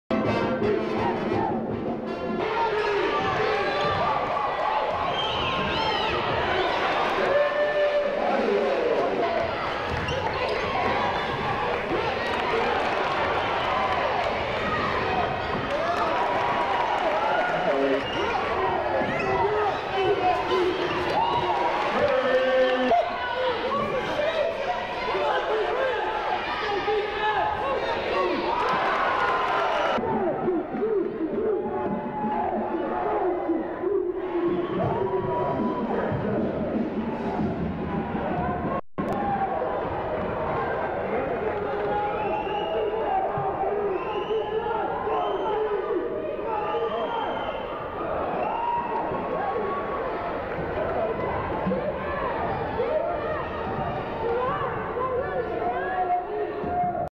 UNLV Lady Rebel Basketball 1992
UNLV Lady Rebel basketball from the South Gym at UNLV. Probably 1992